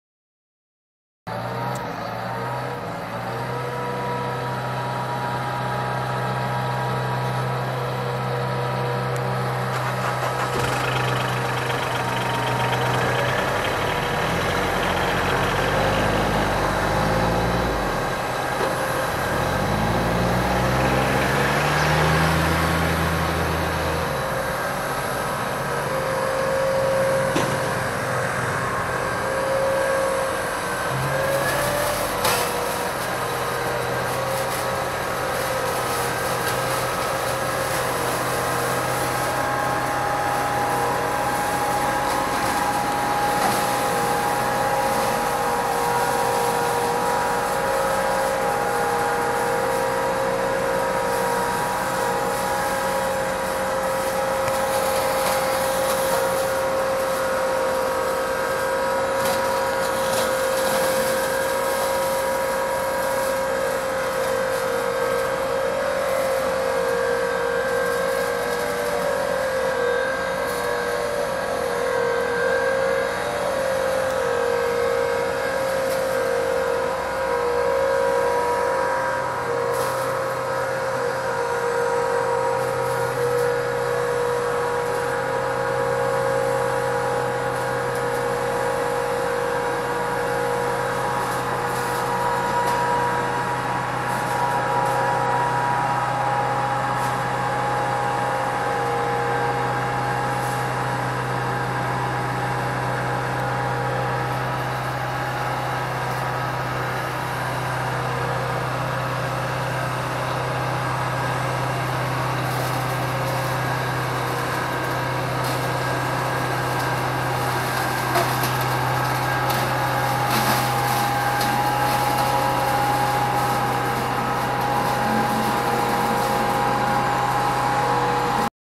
John Deere 6125R met McConnel armmaaier
Een verwilderde houtkant terug zetten met een armmaaier van McConnel gemonteerd op een John Deere 6125R AutoPowr.
De klepelmaaier verbrijzelt direct het afgemaaide materiaal zodat dit nadien niet moet opgeruimd worden. Het versnipperde materiaal breekt af in de berm en zorgt voor natuurlijke bemesting.